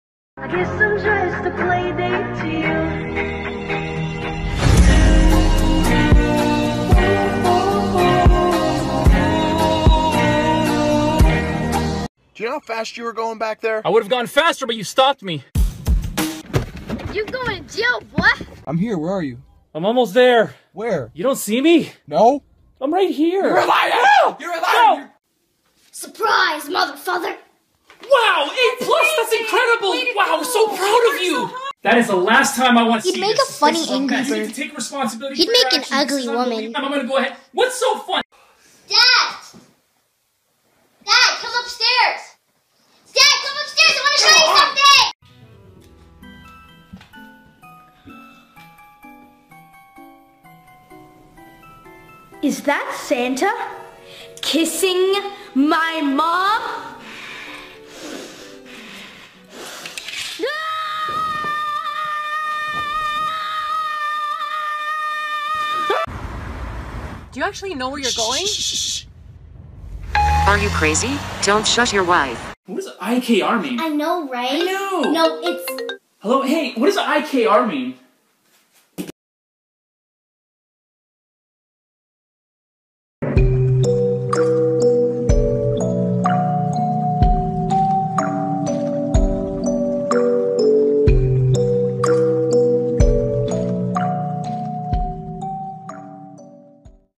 Eh bee family vines Part 1 Gacha life/club
Apps I used:

Gacha life
Gacha club
KineMaster ⏩

Screenshots: 65

Characters: Robert, Mary, P. Dustin, P. Amelia, Police, and Adam (Robert's friend)

Device: Tablet (Samsung Galaxy Tab3V)